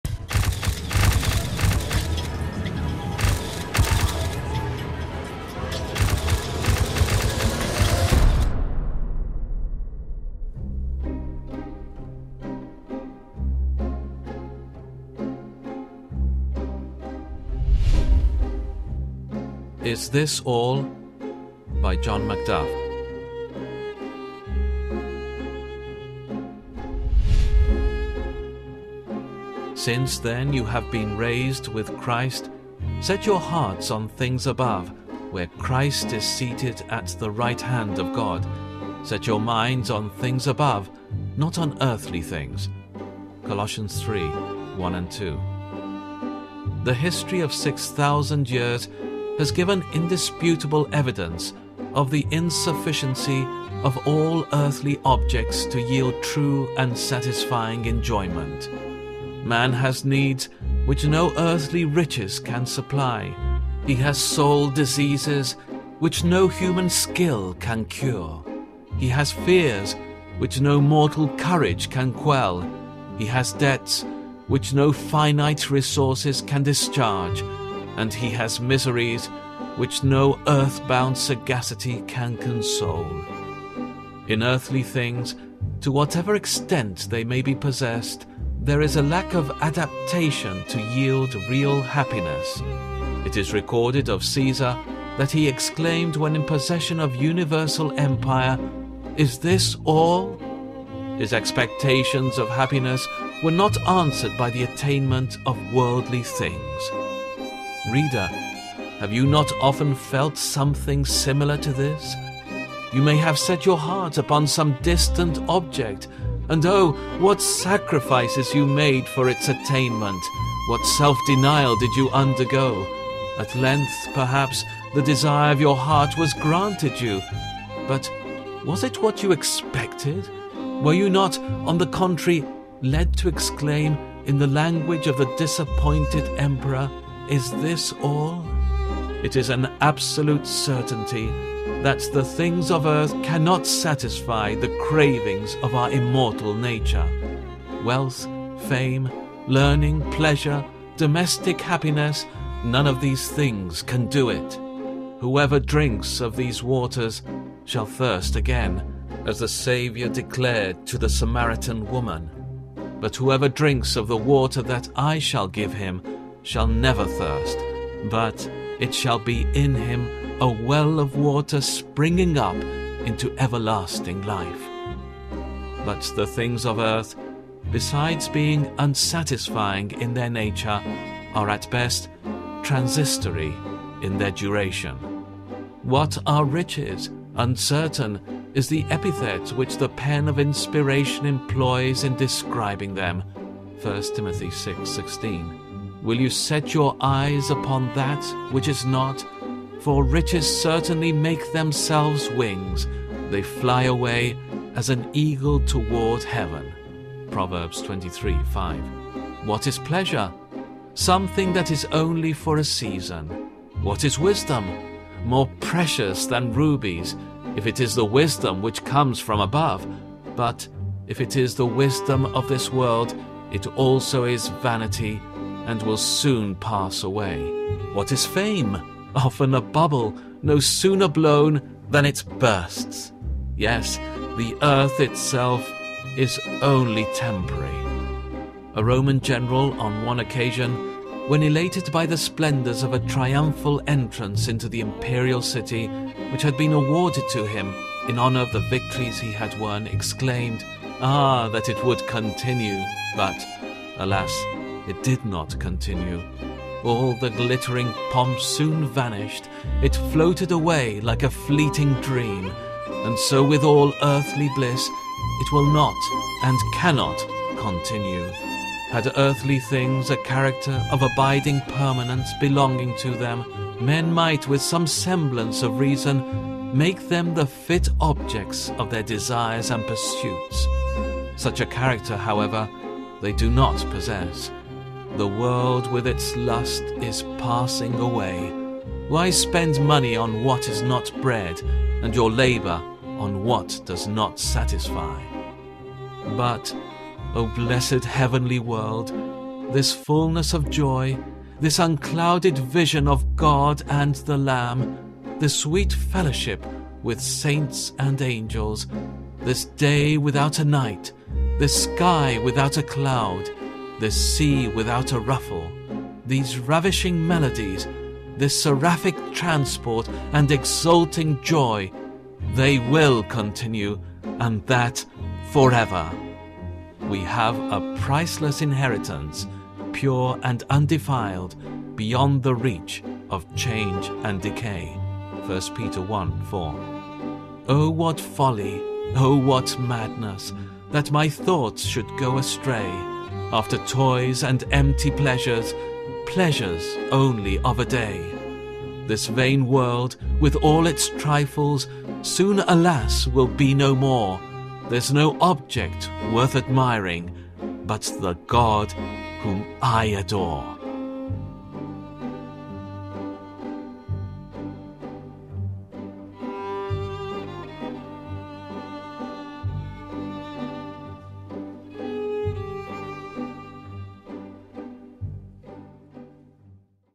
Is this all?
(John MacDuff, "THE FOOTSTEPS OF JESUS" 1856)

"Since, then, you have been raised with Christ — set your hearts on things above, where Christ is seated at the right hand of God. Set your minds on things above — not on earthly things!" Colossians 3:1-2

The history of six thousand years has given indisputable evidence of the insufficiency of all earthly objects to yield true and satisfying enjoyment.
Man has needs — which no earthly riches can supply;
he has soul diseases — which no human skill can cure;
he has fears — which no mortal courage can quell;
he has debts — which no finite resources can discharge; and
he has miseries — which no earth-born sagacity can console.

In earthly things, to whatever extent they may be possessed, there is a lack of adaptation to yield real happiness. It is recorded of Caesar, that he exclaimed, when in possession of universal empire, "Is this all?" His expectations of happiness were not answered by the attainment of worldly things.

Reader, have you not often felt something similar to this? You may have set your heart upon some distant object; and oh! what sacrifices you made for its attainment! What self-denial did you undergo! At length, perhaps, the desire of your heart was granted you. But was it what you expected? Were you not, on the contrary, led to exclaim, in the language of the disappointed emperor, "Is this all?"

It is an absolute certainty, that the things of earth cannot satisfy the cravings of our immortal nature. Wealth, fame, learning, pleasure, domestic happiness — none of these things can do it. "Whoever drinks of these waters shall thirst again," as the Savior declared to the Samaritan woman; "but whoever drinks of the water that I shall give him, shall never thirst — but it shall be in him a well of water springing up into everlasting life!"

But the things of earth, besides being unsatisfying in their nature — are, at best, transitory in their duration.

What are RICHES? 'Uncertain' is the epithet which the pen of inspiration employs in describing them. (1 Timothy 6:16) "Will you set your eyes upon that which is not? for riches certainly make themselves wings; they fly away as an eagle toward heaven!" (Proverbs 23:5)

What is PLEASURE? Something that is only for a season.

What is WISDOM? More precious than rubies, if it is the wisdom which comes from above; but if it the wisdom of this world — it also is vanity, and will soon pass away.

What is FAME? Often a bubble, no sooner blown — than it bursts!

Yes, the earth itself is only temporary!

A Roman general, on one occasion, when elated by the splendors of a triumphal entrance into the imperial city, which had been awarded to him in honor of the victories he had won, exclaimed, "Ah, that it would continue!" But, alas! it did not continue. All the glittering pomp soon vanished! It floated away like a fleeting dream. And so with all earthly bliss — it will not, and cannot, continue. Had earthly things a character of abiding permanence belonging to them, men might with some semblance of reason, make them the fit objects of their desires and pursuits. Such a character, however, they do not possess. "The world with its lust is passing away!" "Why spend money on what is not bread, and your labor on what does not satisfy?"

But, O blessed heavenly world!
This fullness of joy;
this unclouded vision of God and the Lamb;
this sweet fellowship with saints and angels;
this day without a night;
this sky without a cloud;
this sea without a ruffle;
these ravishing melodies;
this seraphic transport and exulting joy
 — they will continue — and that forever!
"We have a priceless inheritance — pure and undefiled, beyond the reach of change and decay!" (1 Peter 1:4)

"O what folly, O what madness!
 That my thoughts should go astray,
 After toys and empty pleasures —
 Pleasures only of a day!

 This vain world, with all its trifles,
 Soon, alas! will be no more;
 There's no object worth admiring,
 But the God whom I adore!"